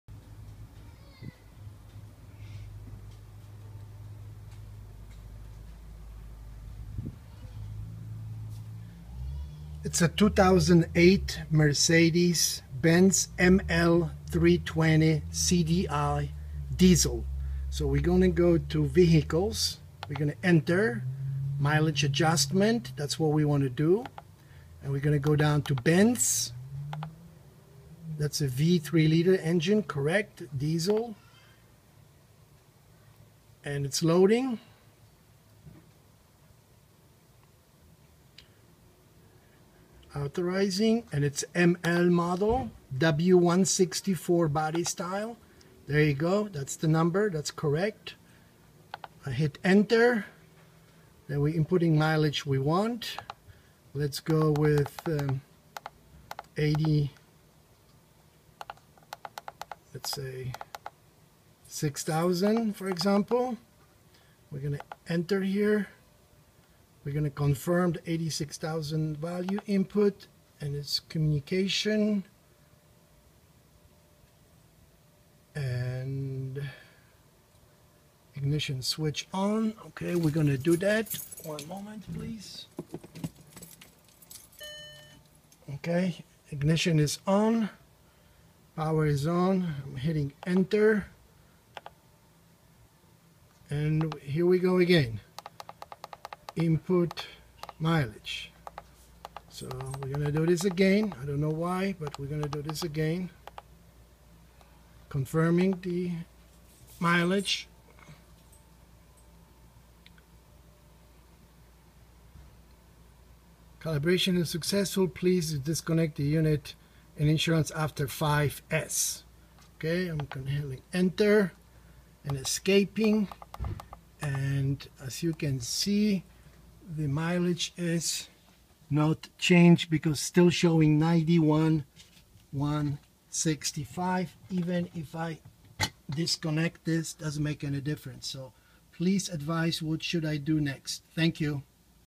how to use obdstar x300m do odometer adjustment
OBDSTAR X300M Special for Odometer Adjustment and OBDII,The most devoted, the most professional X300 M is only for odometer adjustment, which creates profession. The main unit system adopts ARM high speed chip to keep it running fast, stable and anti-jamming.